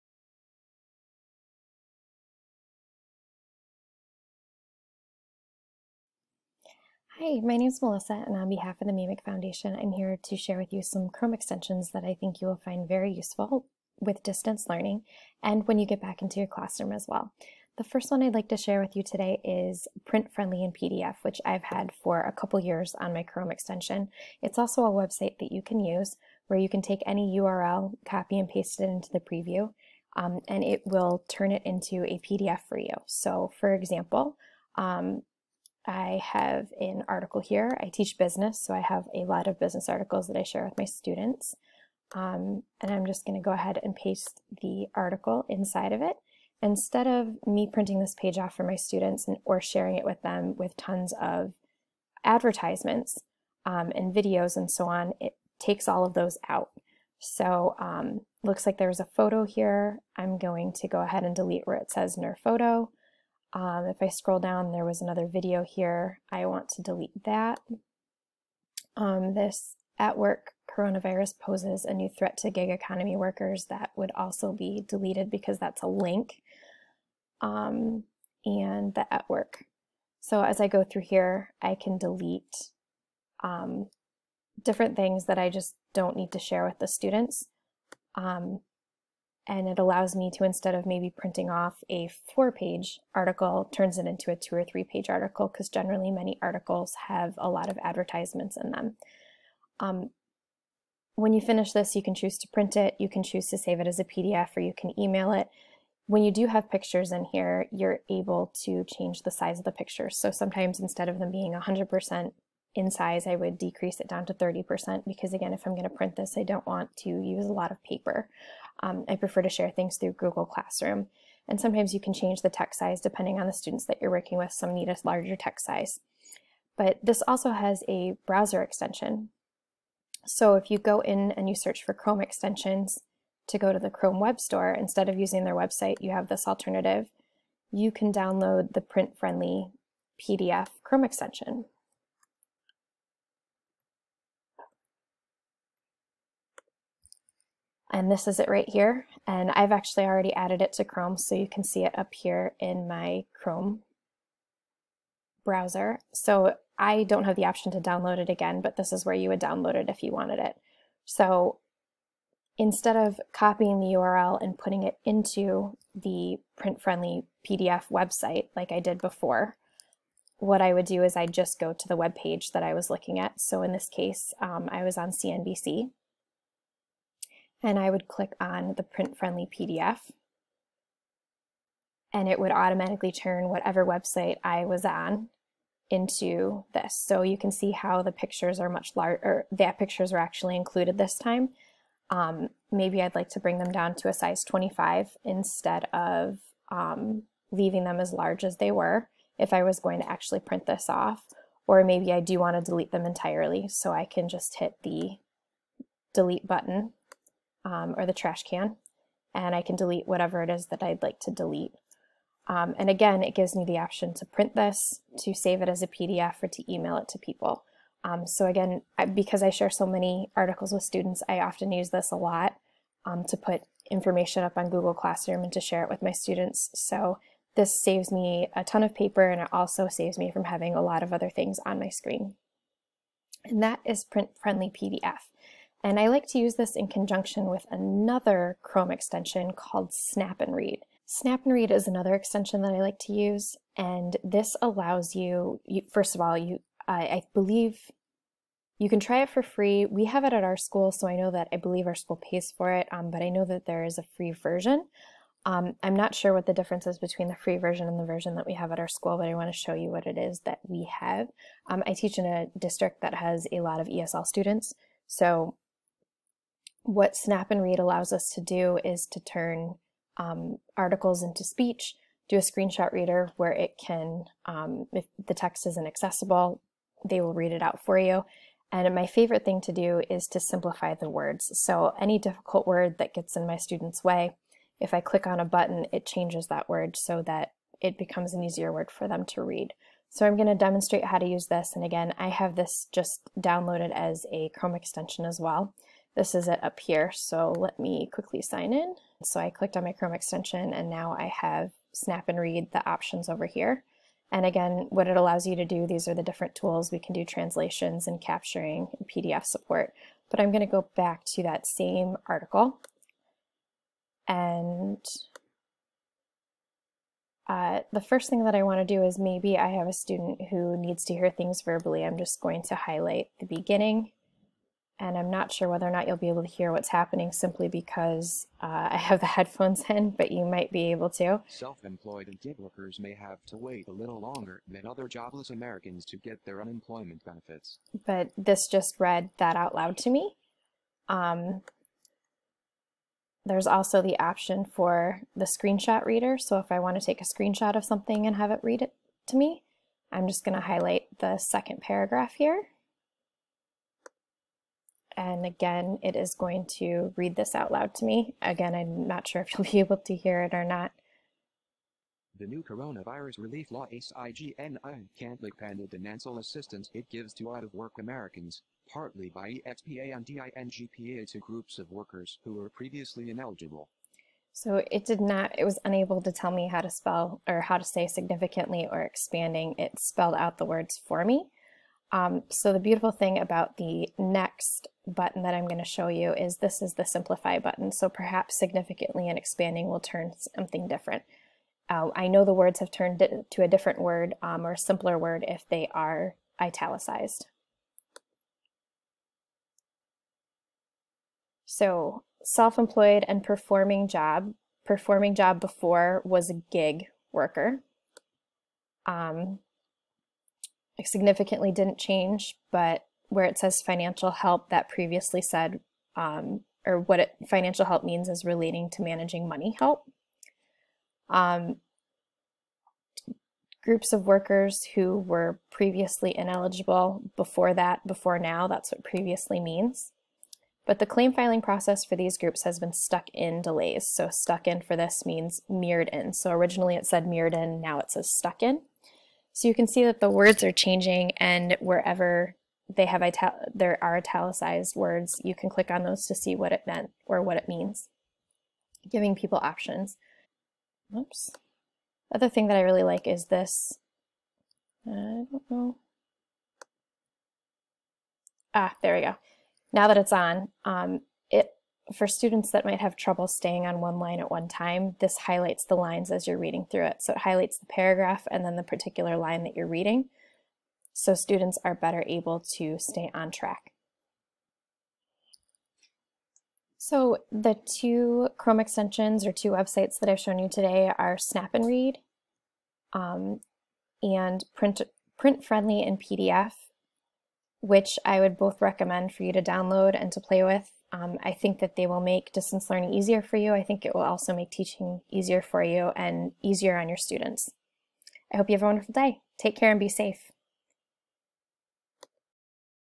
“Print Friendly & PDF” and “Snap & Read” Chrome Extensions
High school teacher and Meemic family member Melissa Gordon walks teachers through two great resources for teachers to help with online learning: “Print Friendly & PDF,” which creates handy, stripped-down versions of websites for easy distribution; and “Snap & Read,” which is especially helpful for ESL students.